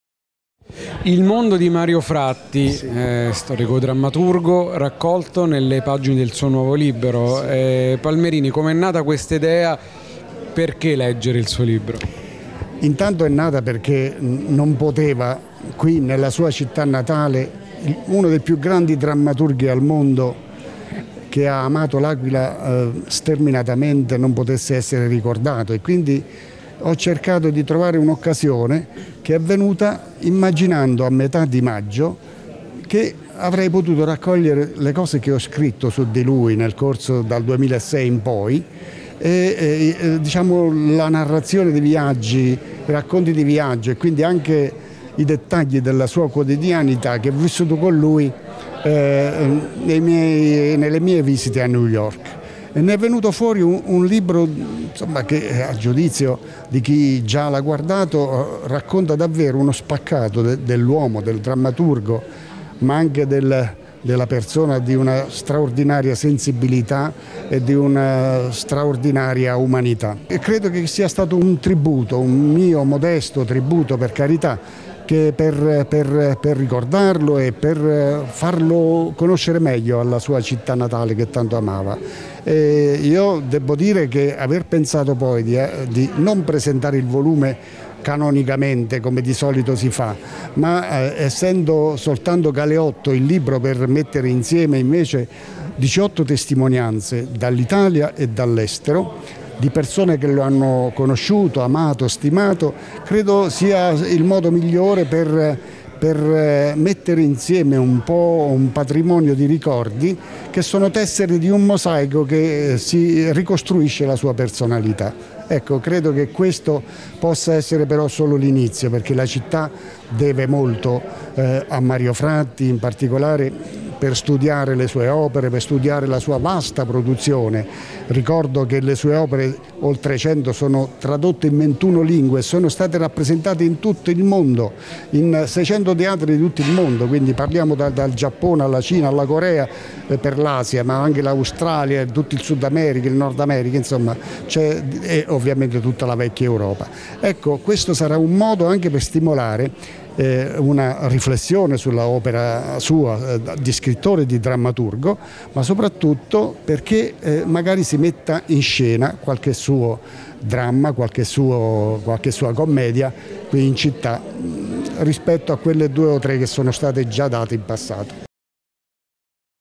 GOFFREDO PALMERINI PRESENTA "IL MONDO DI MARIO FRATTI"
PRESENTATO A L'AQUILA IL NUOVO LIBRO DI GOFFREDO PALMERINI, DAL TITOLO  "IL MONDO DI MARIO FRATTI"